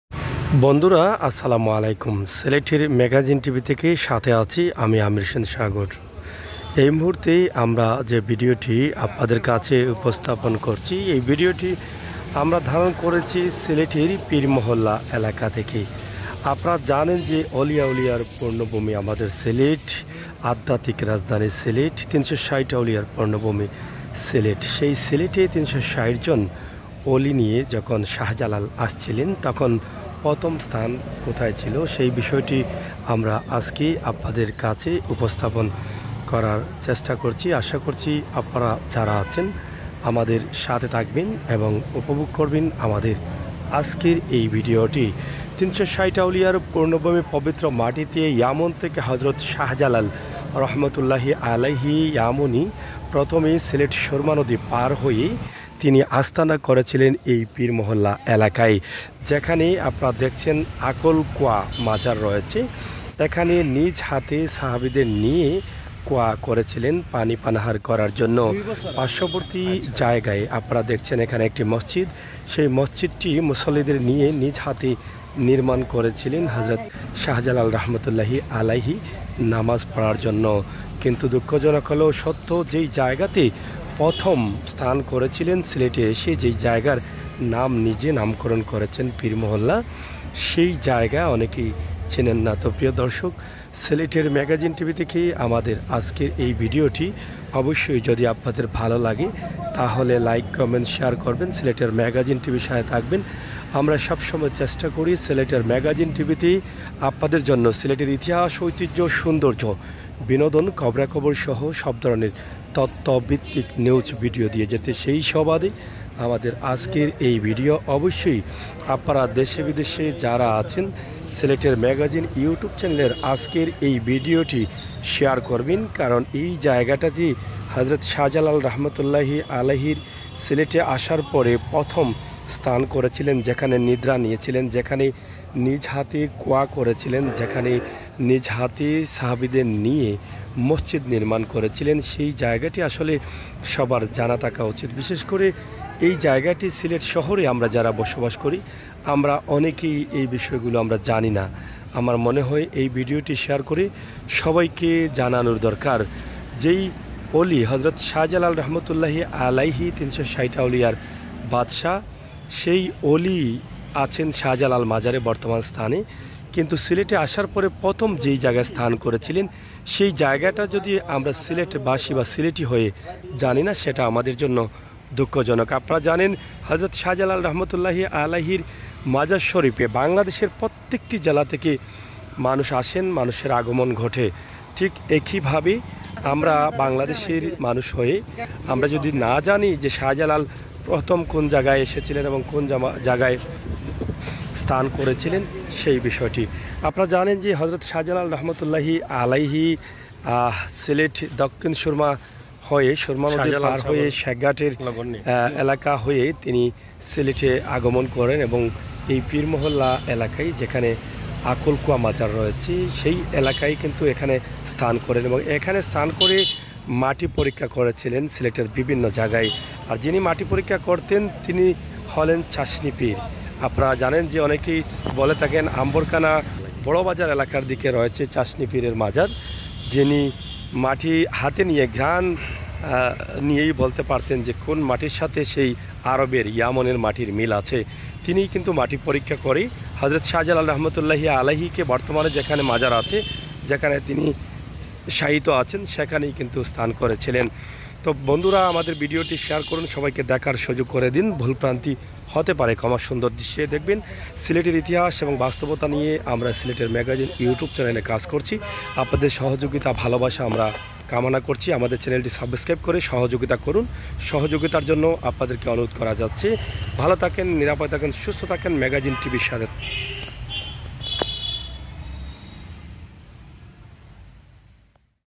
সিলেটে প্রথম মসজিদ নির্মাণ ও পানি খাওয়ার প্রথম খোয়া,শাজলালের নিজ হাতে তৈরী করেছিলেন,সেই পবিত্র মাটি
সিলেটে প্রথম মসজিদ নির্মাণ ও পানি খাওয়ার প্রথম খোয়া,শাজলালের নিজ হাতে তৈরী করেছিলেন,পীরমহল্লায়